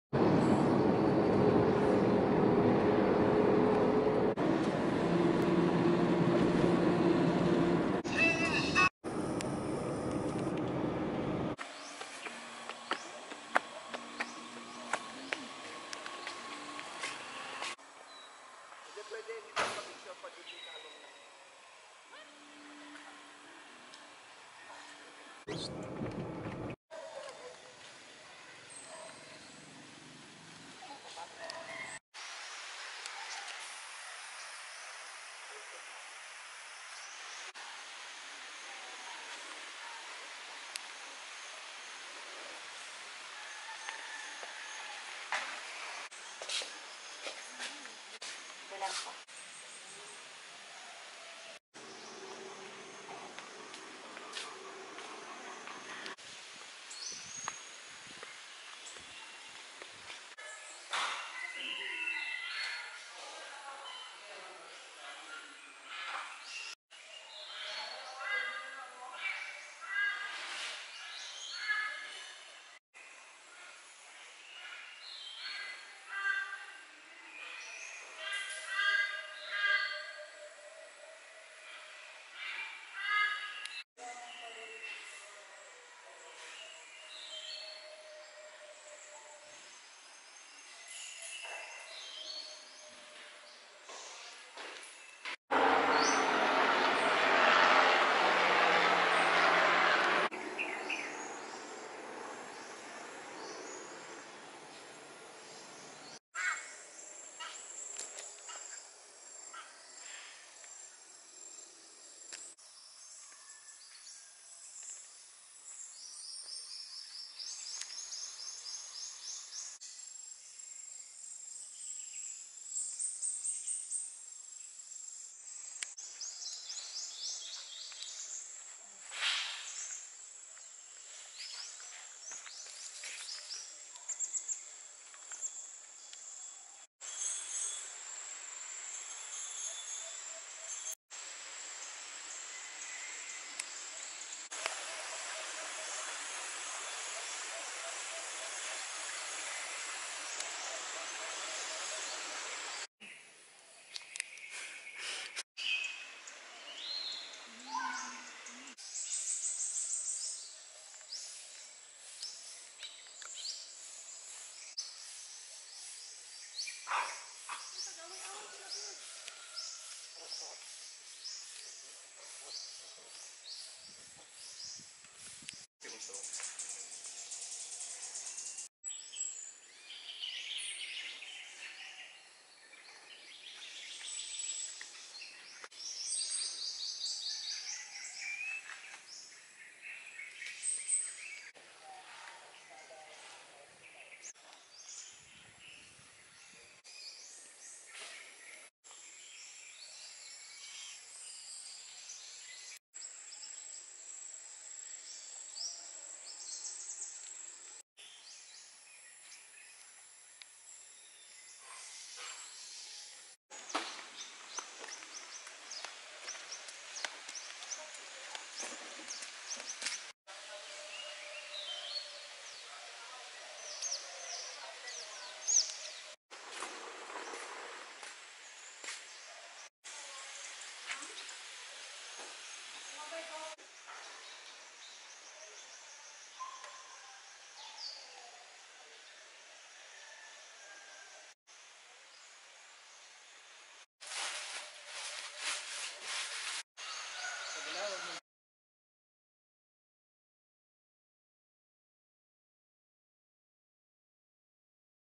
Davao vlog | Philippine Eagle Foundation
Part 1 of nature trip 🍃 First time to take a break since the pandemic hit! Refreshing but I need about 700 more days like this T_T